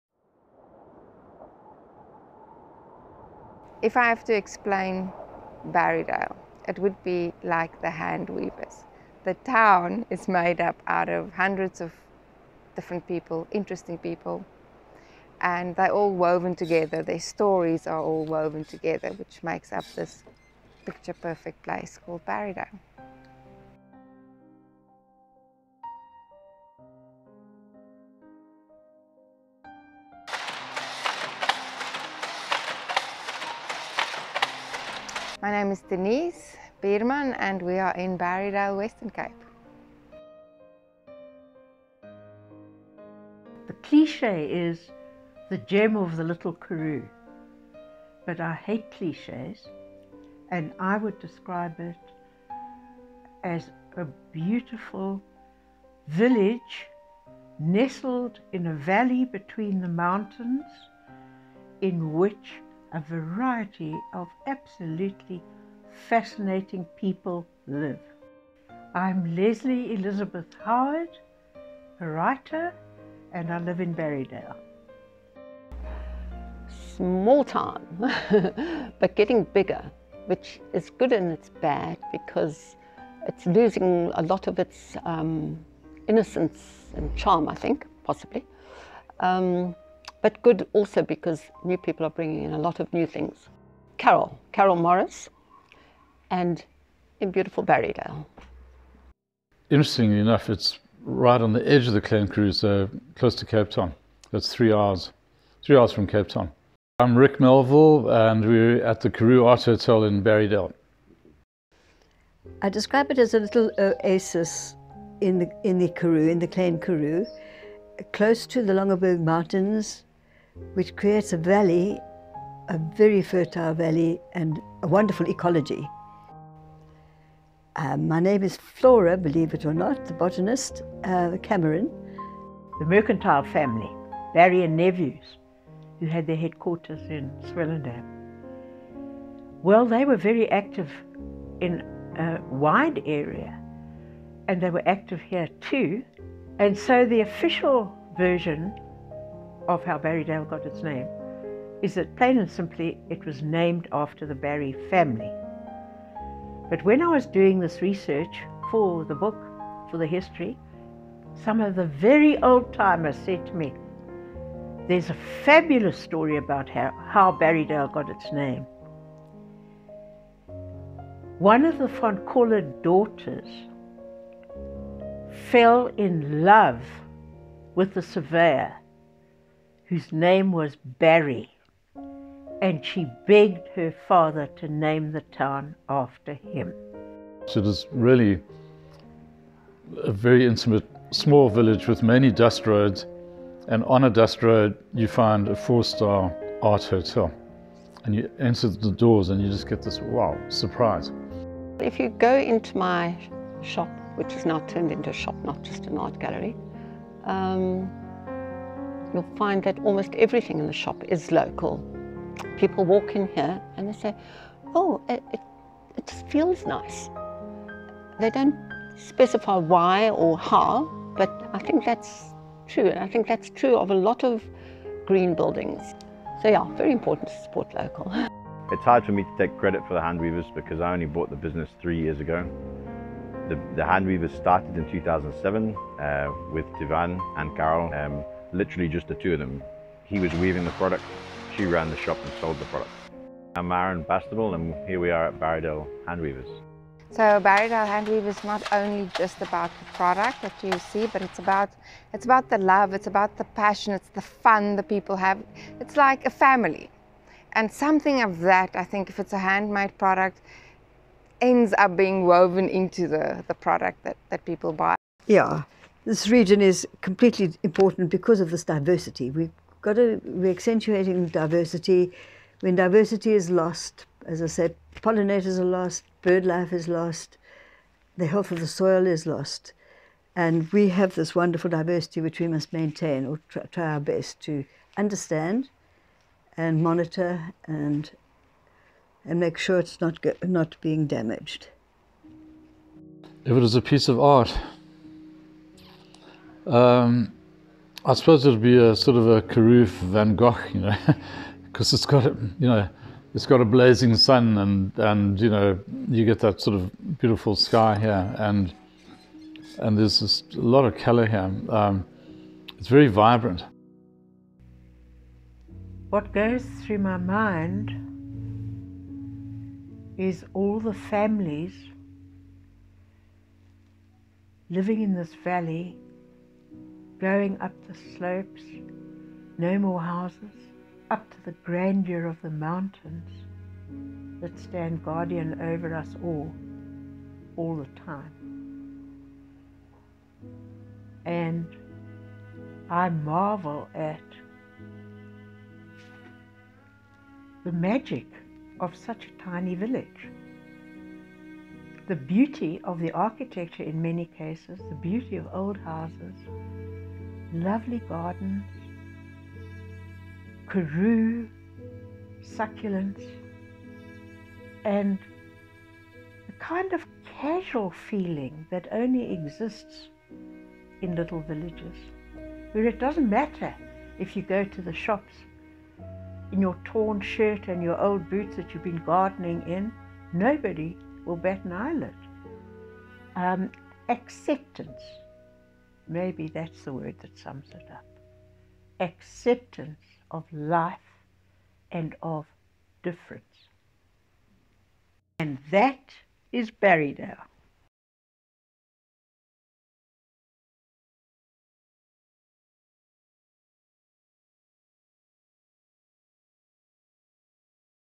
Barrydale, Western Cape South Africa
Barrydale is a village located on the border of the Overberg and Klein Karoo regions of the Western Cape Province in South Africa. Named after Joseph Barry, a well known merchant of the 19th century it is situated at the northern end of the Tradouw's pass which winds its way through the mountains to Swellendam.
Barrydale's history dates back to the early 18th century when farmers moved into the area looking for fertile arable land with water. The community built their church on a spot where the R62 and R324 roads meet. In the days before the church was built there were a number of nagmaal houses (houses where Holy Communion could be celebrated) and a school, but not much else. The Dutch Reformed Community of Barrydale came into being in 1878 when land was purchased to build the church.

As the farmers in the area were encouraged to plant vineyards and orchards, it was natural that a winery and distillery would eventually be built. In 1940 the Barrydale Koöperatiewe Wynkelder was formed and a distillery established giving rise to the wine industry in the area. Joseph Barry Brandy, produced locally, was voted best brandy in the world in 2003.

Over the years the village grew and eventually a municipality was established in 1921. Today there is an estimated population of ~4100 permanent residents. The population increases dramatically in the tourist season, with visitors drawn by arts and crafts displays including textiles, jewellery and African souvenirs.

0:00 Barrydale Handweavers
0:44 Leslie Howard
1:16 MUD
1:39 Karoo Art Hotel
1:53 Flora Cameron https:/
2:12 Swellendam Tourism